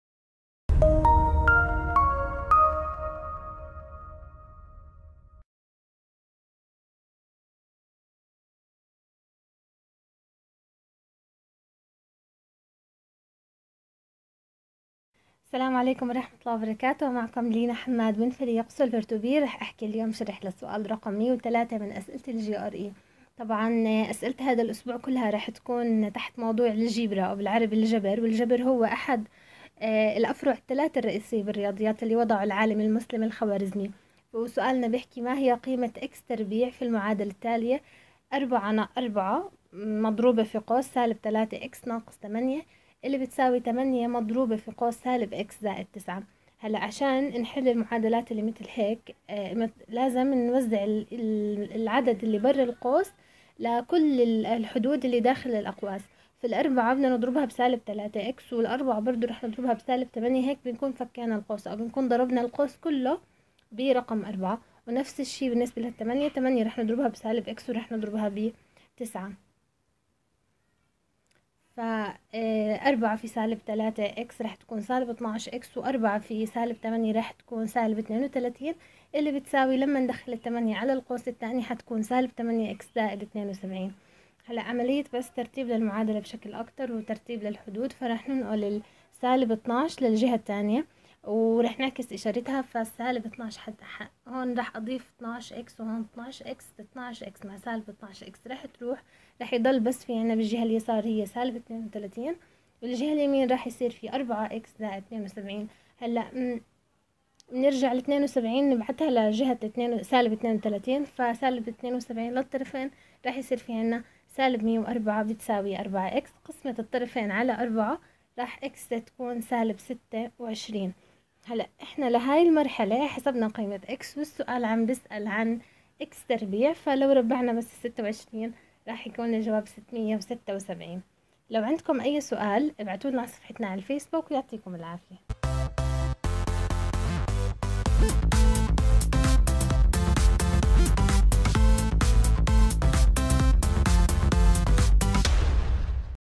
GRE 103
تقديم : لينا حماد
حل السؤال رقم 103 من اسئلة ال GRE